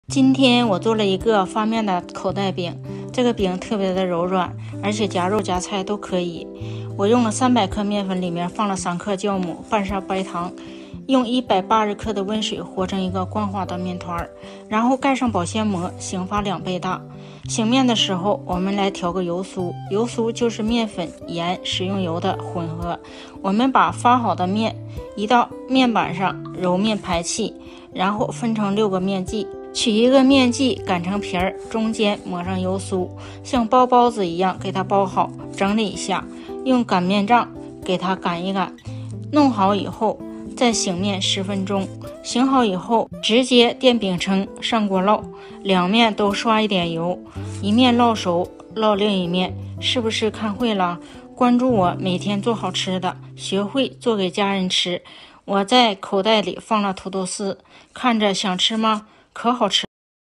Yummy pocket pancakes, especially soft
All-purpose pocket pancakes, especially soft, can be used with meat and vegetables.